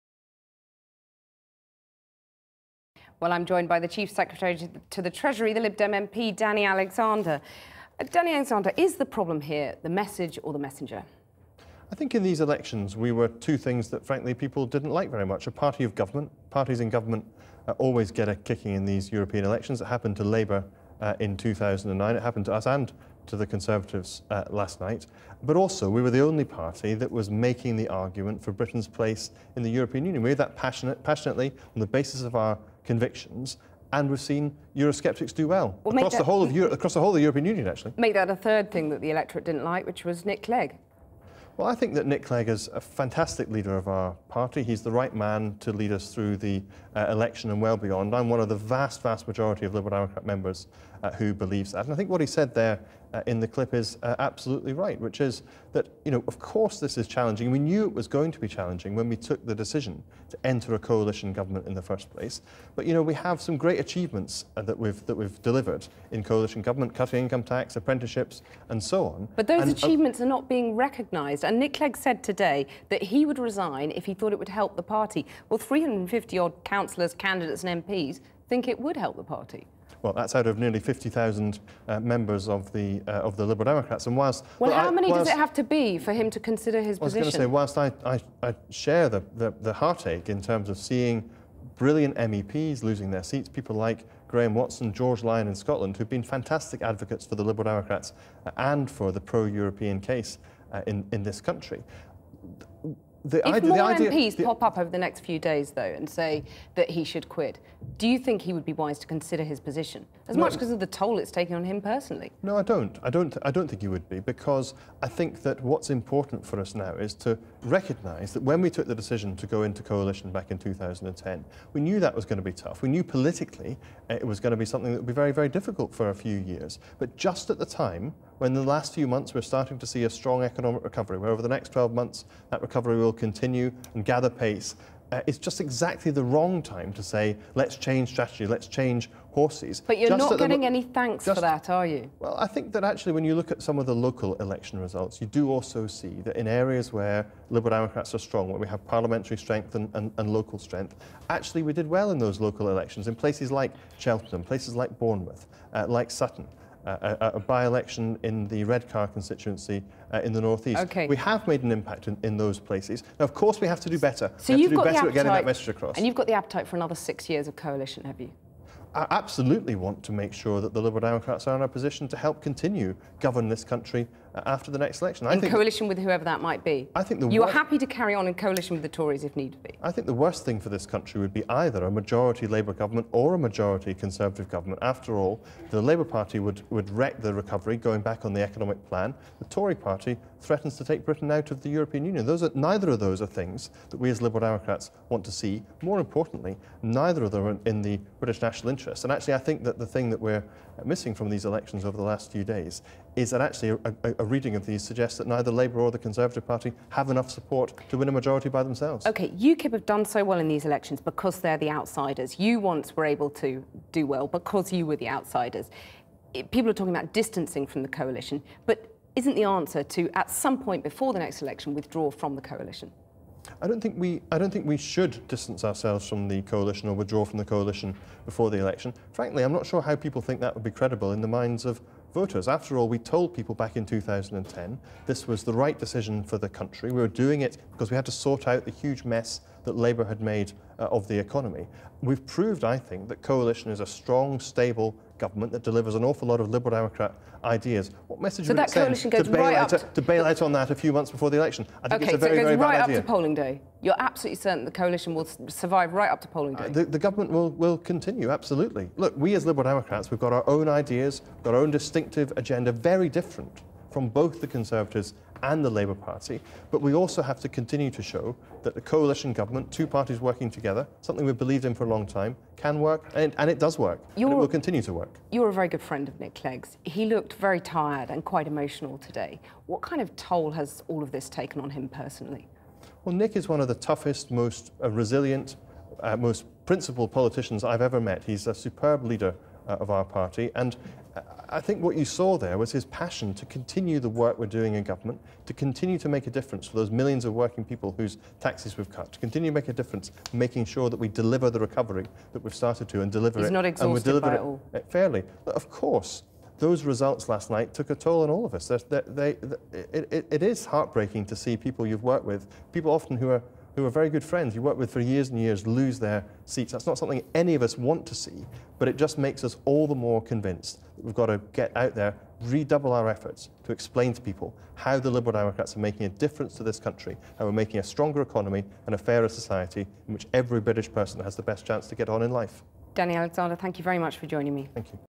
Danny Alexander: governing parties 'always take a kicking'
The Liberal Democrat MP Danny Alexander says parties in government "always take a kicking" in the European elections, and says Nick Clegg looked tired on Monday because of his "passion" to contine Lib Dem work.Sign up for Snowmail, your daily preview of what is on Channel 4 News, sent straight to your inbox,